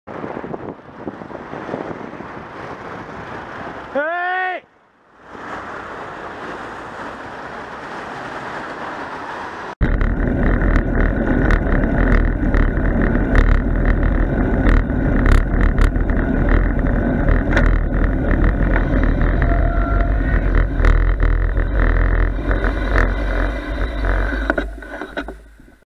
BF60YXO changes lane
It's only a cyclist he'll just have to brake and let me in, not.

Monday 16th February 2015 approx 17:30